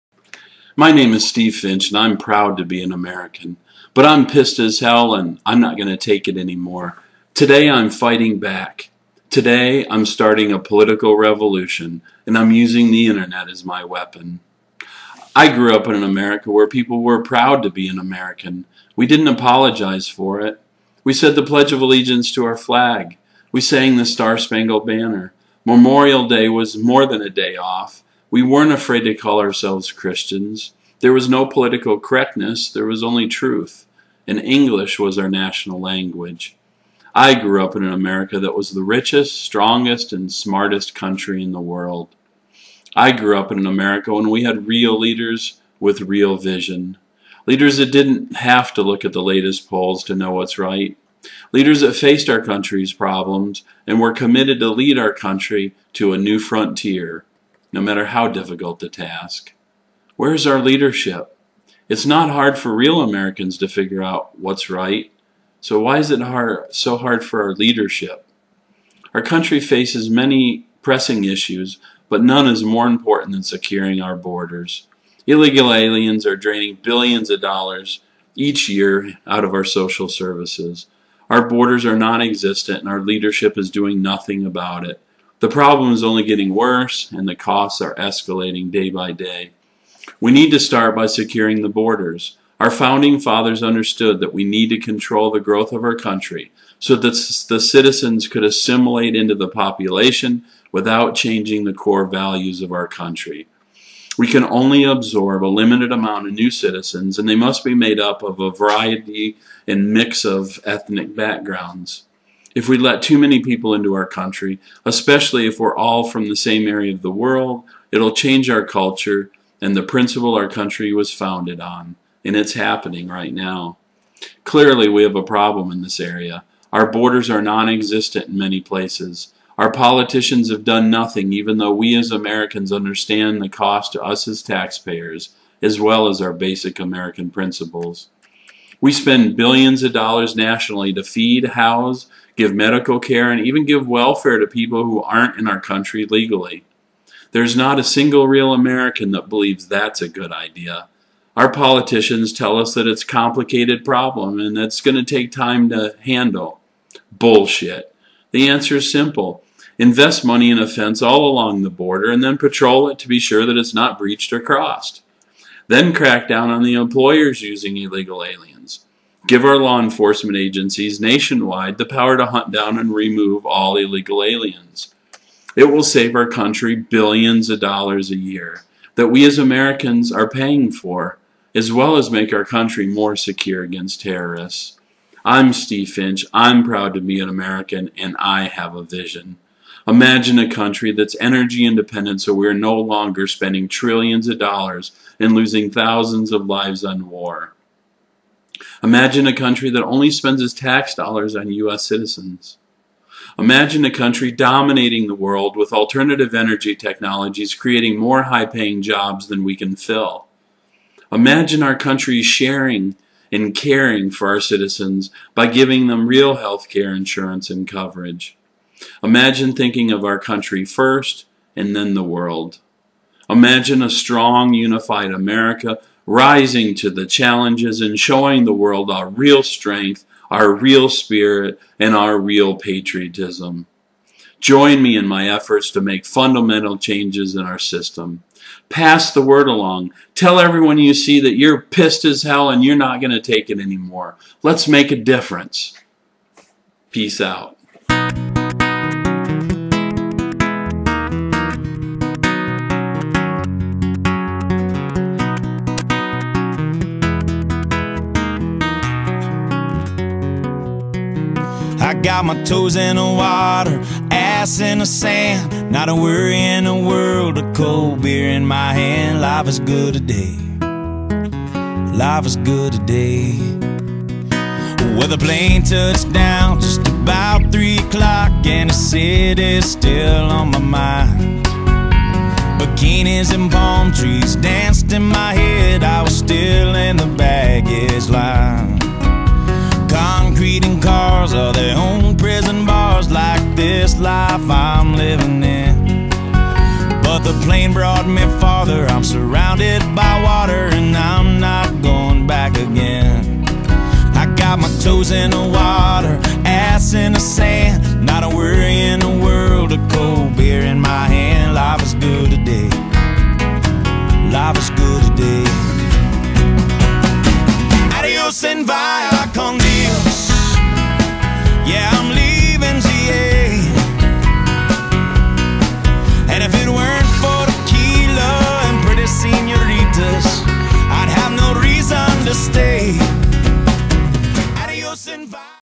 Imagration video.wmv
Real Americans know how much illegal aliens are costing us as taxpayers.  Is it fair we spend our money, our taxes on people who are not in this country legally?  Real Americans say no!!  It is time for our leadership to get tough on this issue.  We need to close the borders, protect them, crack down on the companies using them and give the law enforcement the support to track down the illegal aliens.  We have an immigration policy for a reason.  our founding Fathers knew how important it is,  Watch this video and pass it on.